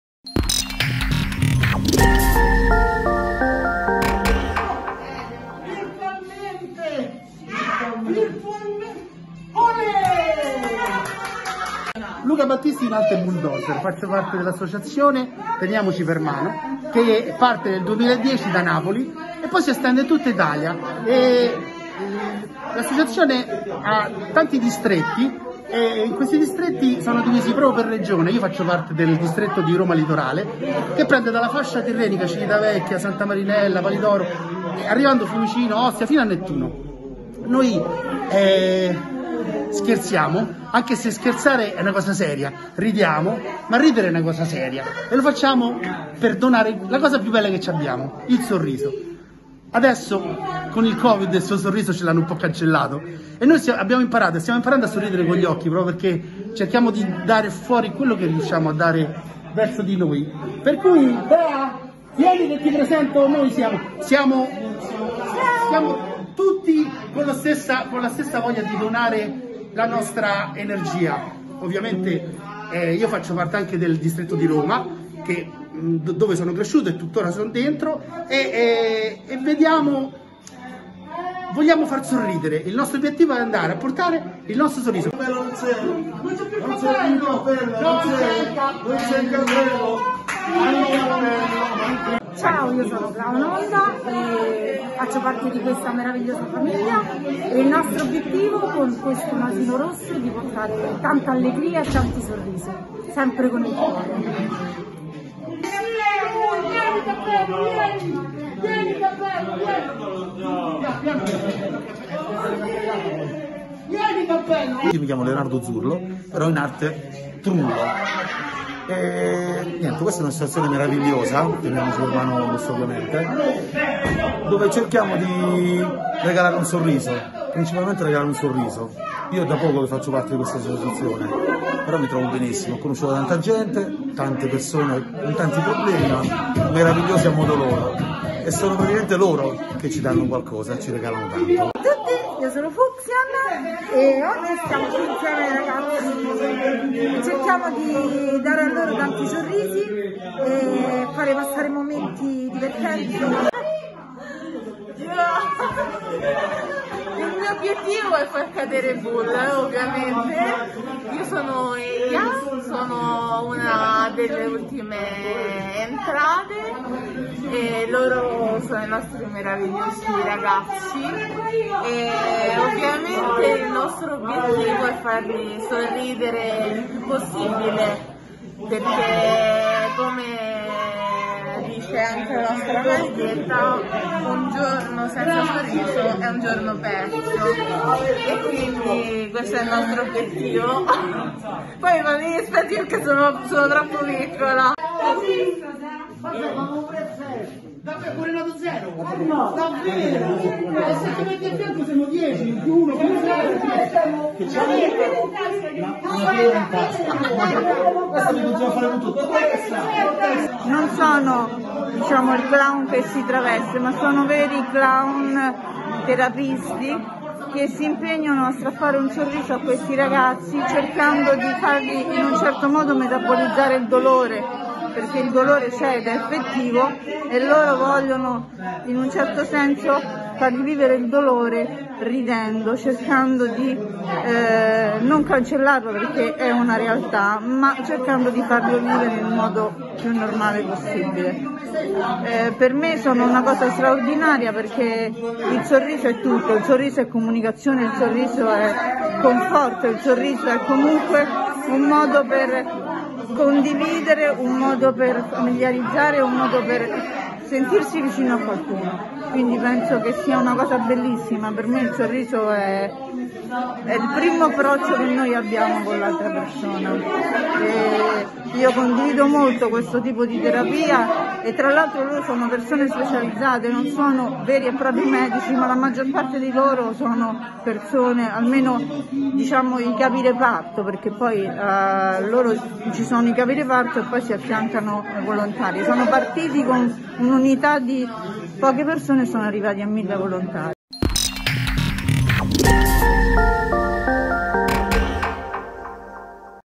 Fiumicino, sorrisi e divertimento con la clownterapia per i ragazzi d'Insieme con i Disabili Onlus
Pomeriggio all'insegna della clownterapia per i ragazzi del centro "Insieme con i Disabili Onlus" di Fiumicino, che si sono divertiti e hanno sorriso grazie all'apporto dei volontari di "Teniamoci per Mano Onlus".